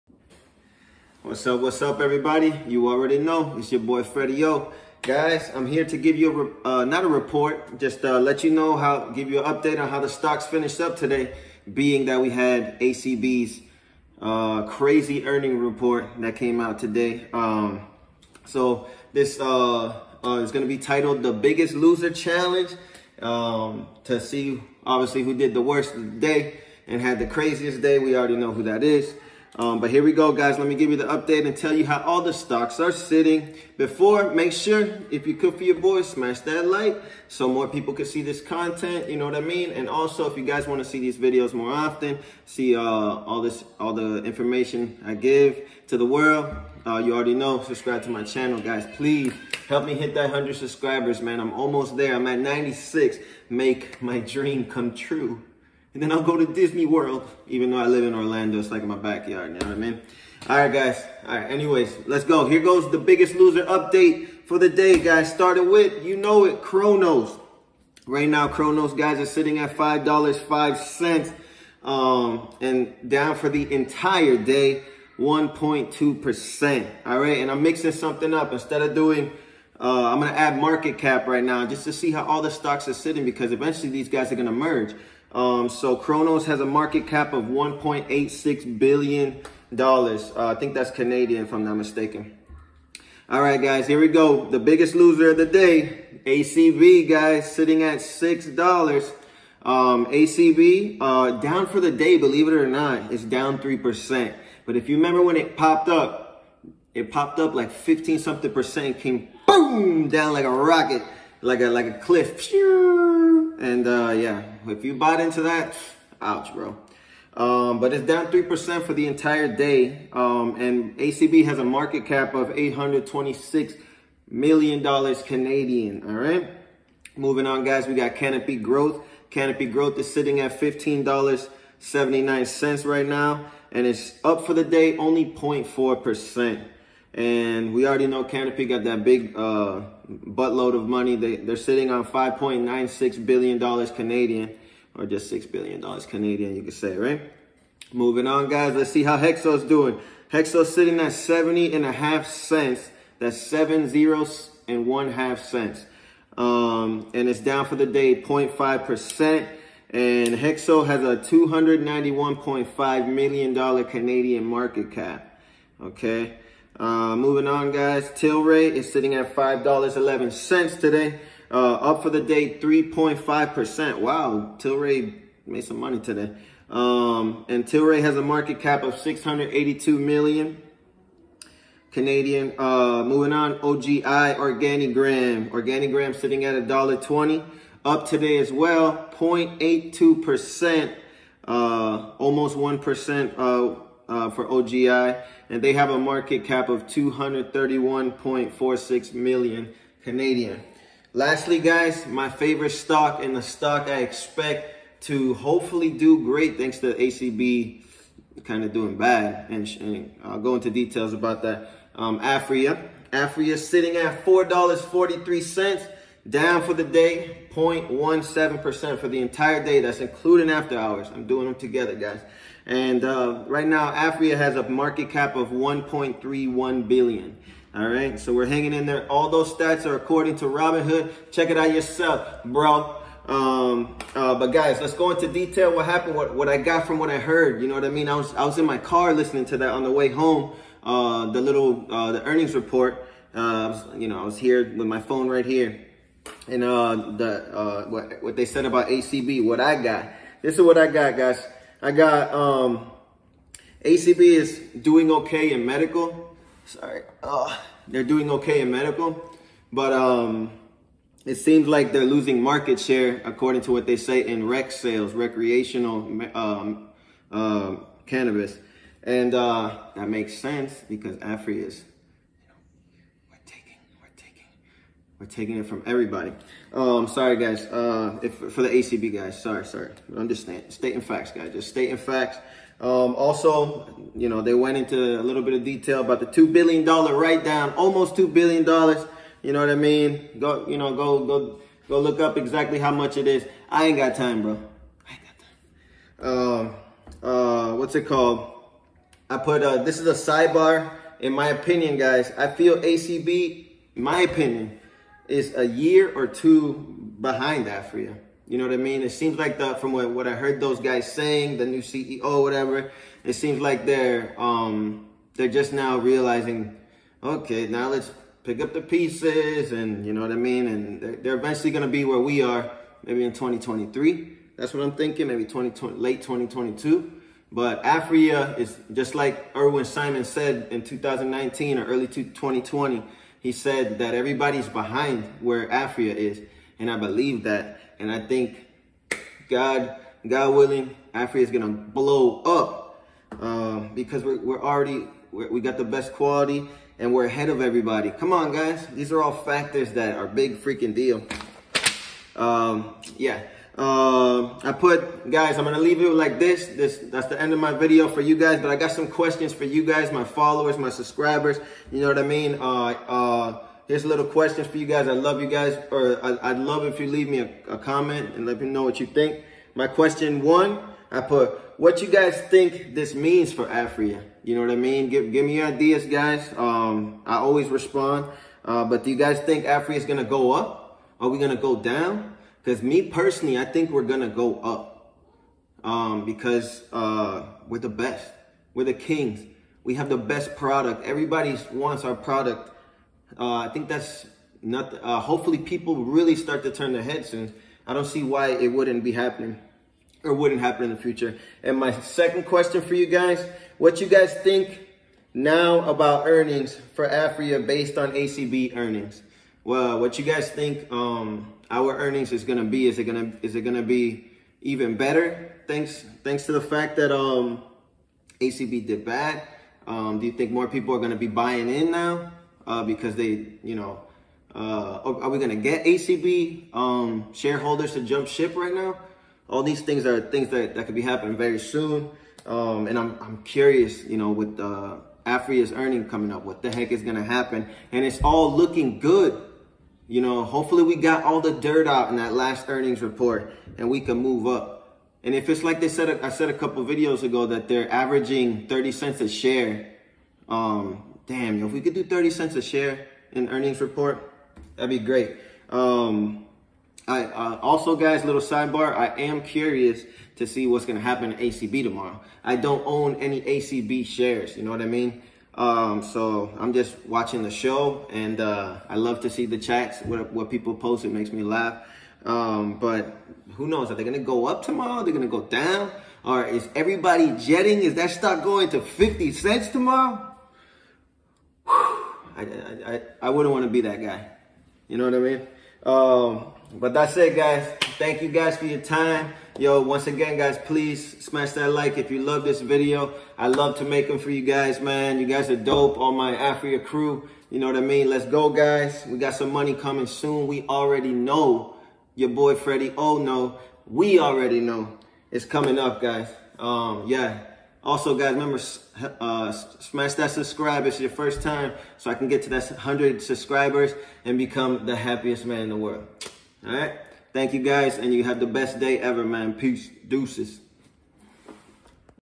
Aphria and the Biggest Loser Of The Day!!!!
Plus stay till the end for some questions I would love feedback pertaining to Aphriainc. all my opinion 😎😎